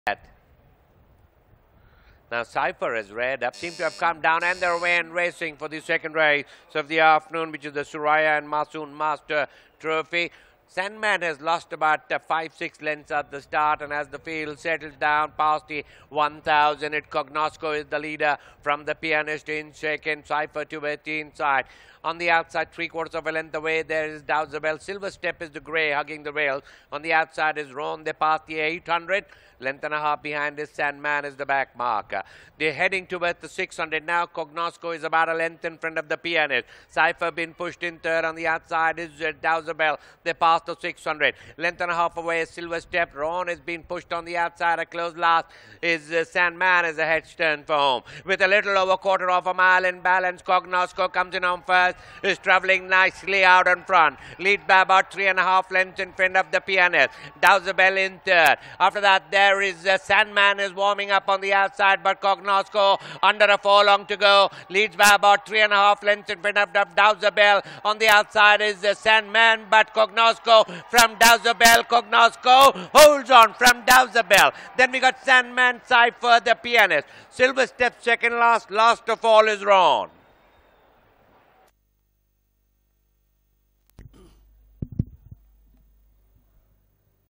COGNOSCO with A Sandesh up wins The Suraiya & Masoom Master Trophy 2022
COGNOSCO  trained by  S Waheed  with A Sandesh  up wins The Suraiya & Masoom Master Trophy 2022  at the Royal Western India Turf Club